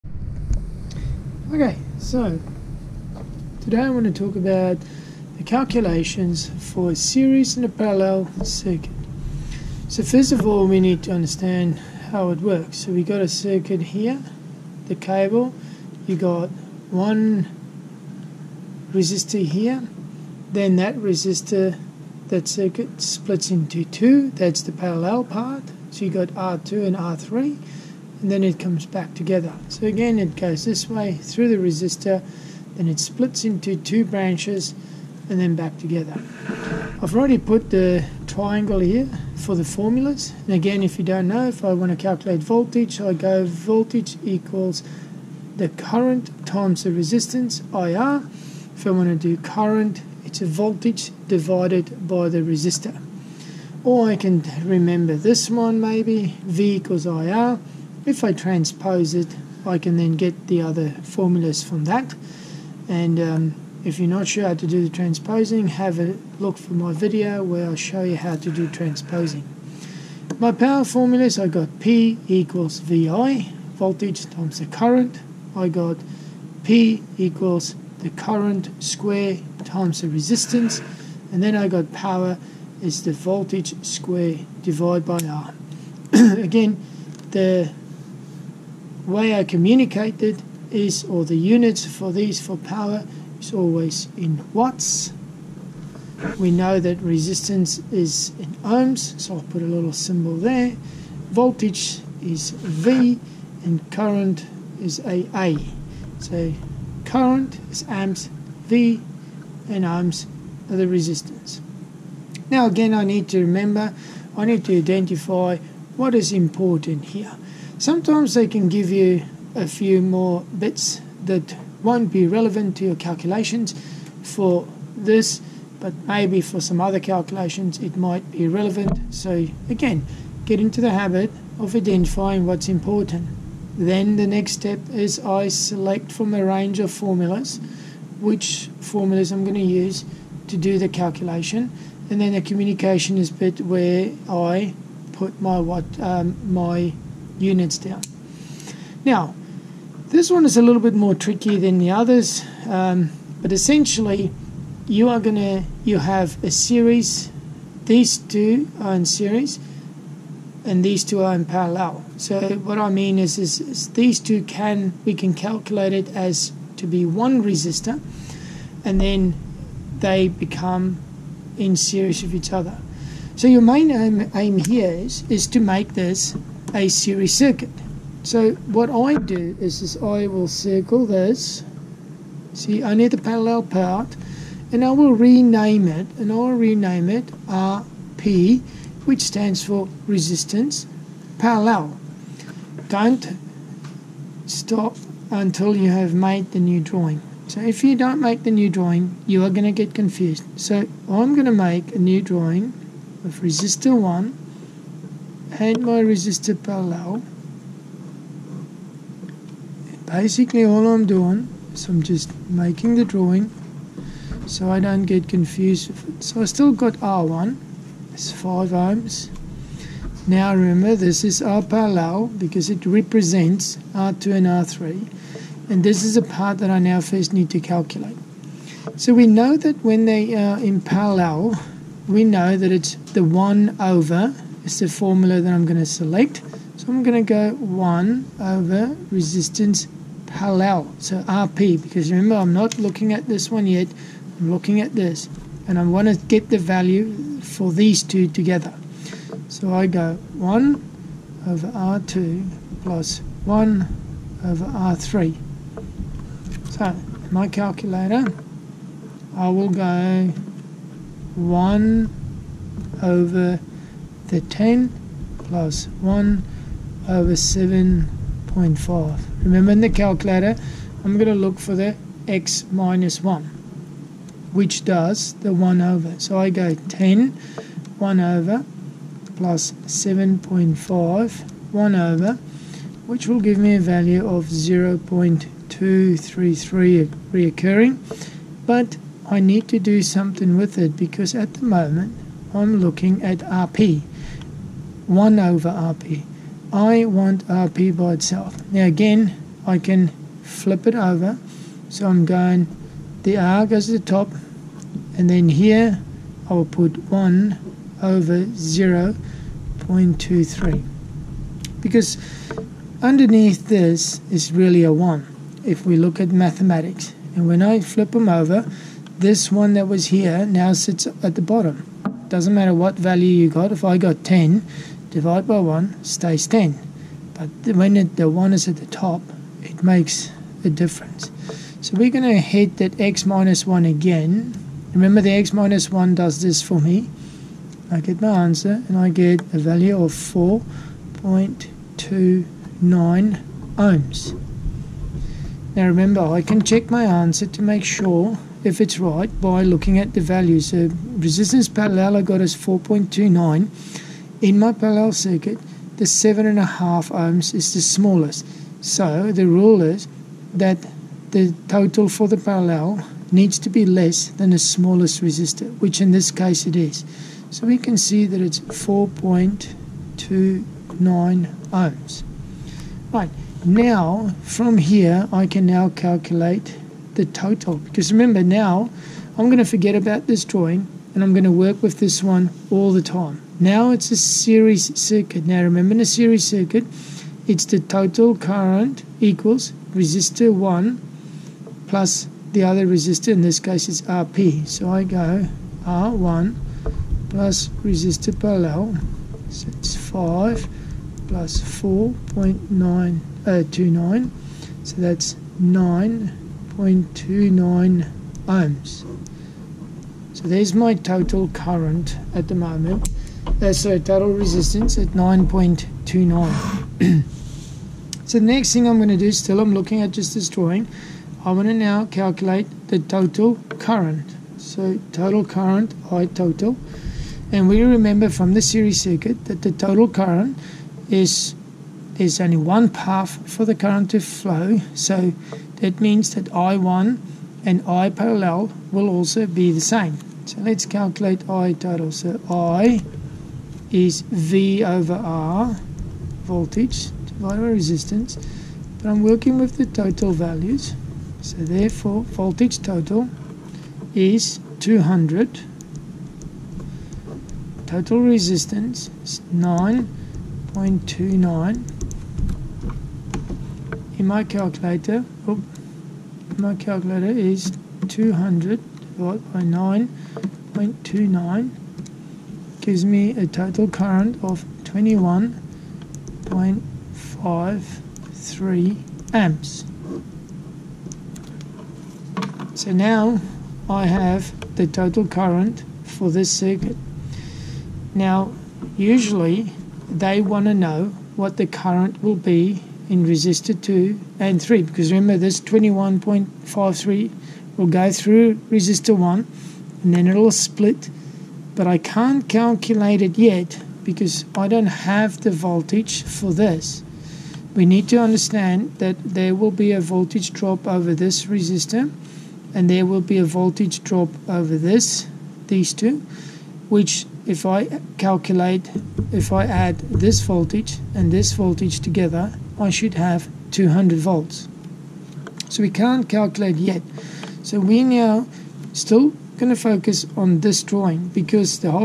Series/parallel circuit calculations.
In this video, I will show you how to do the calculations for a series/parallel circuit.